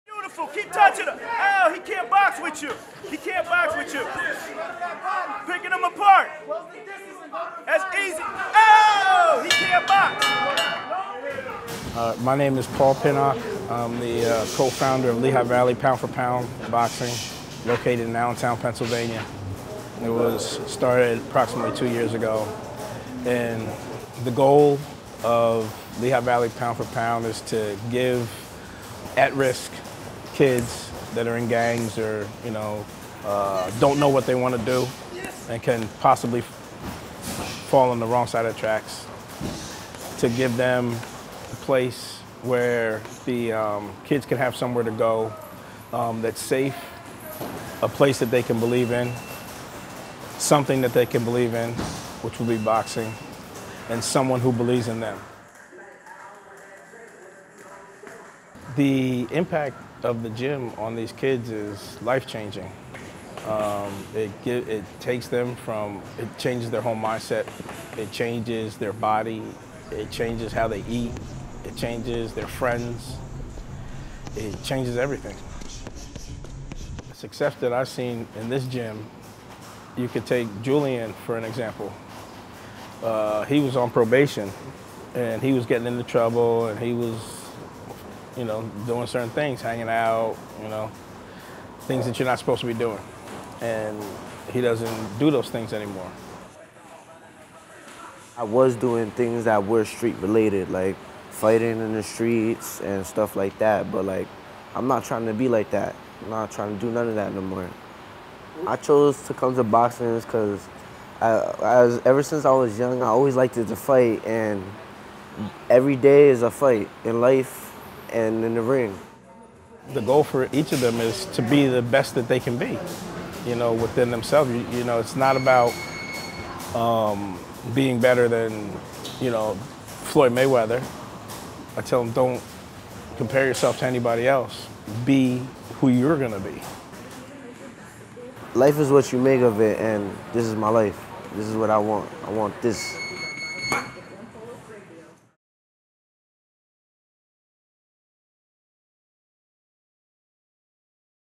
Lehigh Valley Pound 4 Pound Boxing is giving kids a fighting chance to stay off the streets
Paul Pinnock and his Lehigh Valley Pound 4 Pound Boxing gym is giving at-risk kids a fighting chance in Allentown.  Youth learn not only how to box, but also how to steer clear of the lure of the streets. (Video by Saed Hindash | For lehighvalleylive.com)